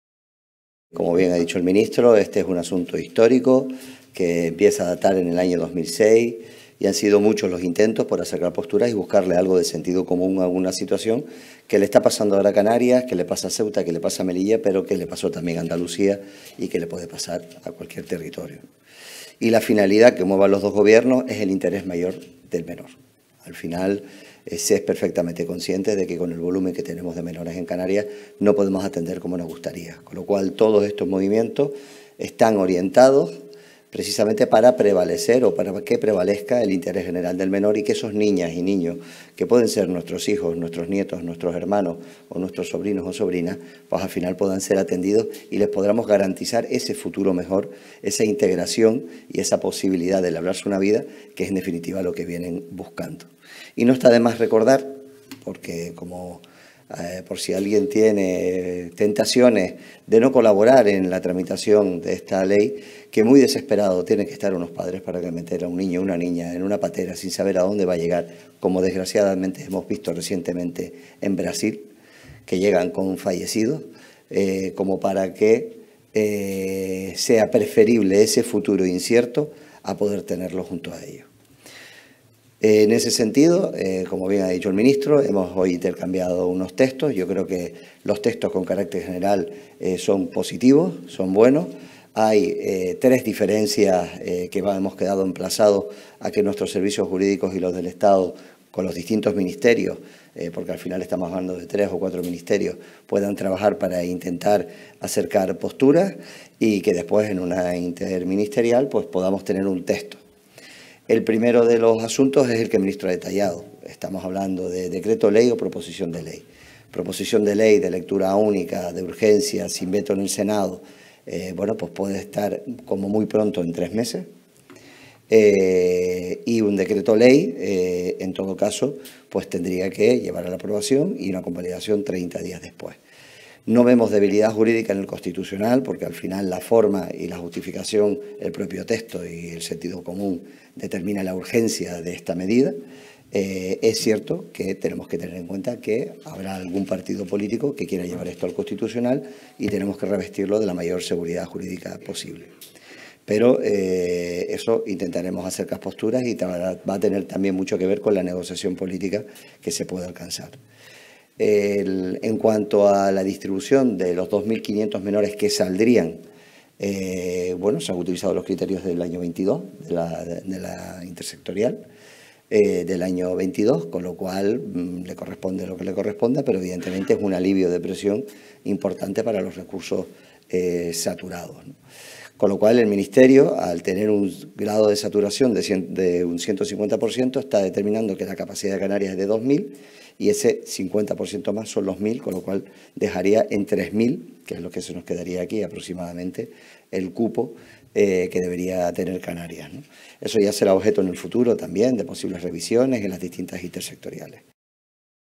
Canarias acudirá al Tribunal Constitucional para defender sus competencias en materia de Costas
Canarias acudirá al Tribunal Constitucional para defender sus competencias plenas en materia de Costas